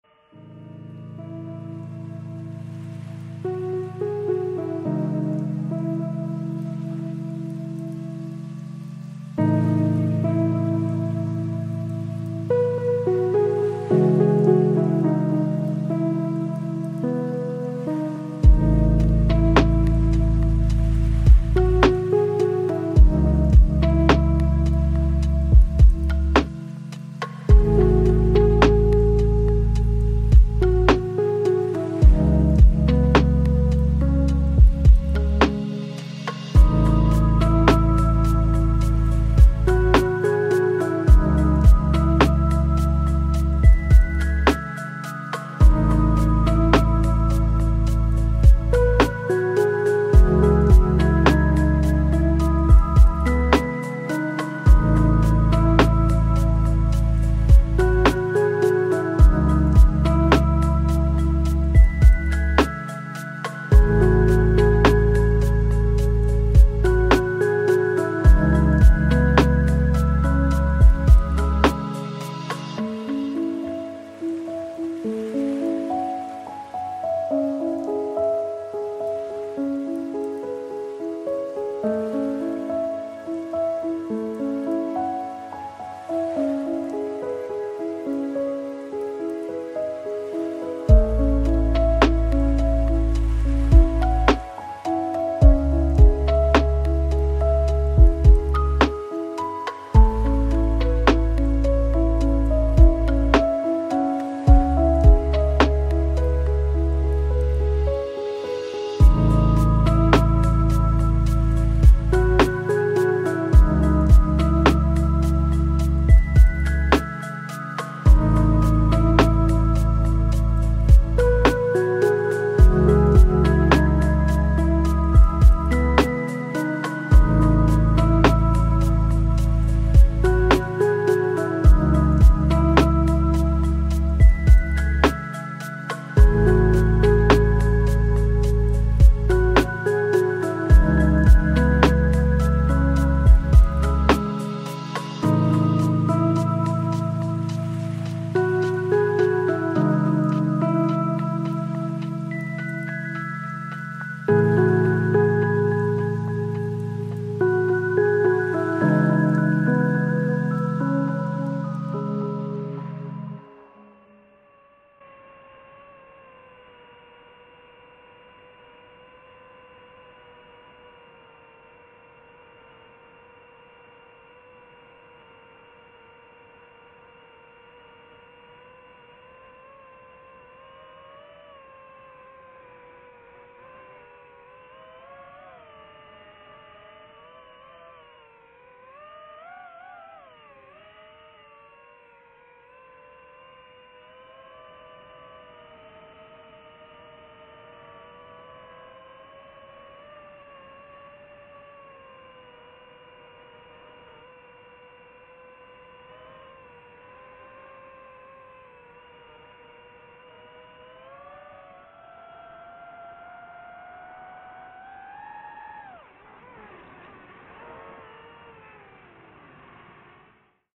Lavas short trail 4K FPV drone video
Short 4x4 trail on the lavas. Seems like it took the better part of an hour in my Suzuki Samurai. But only 3 minutes by drone!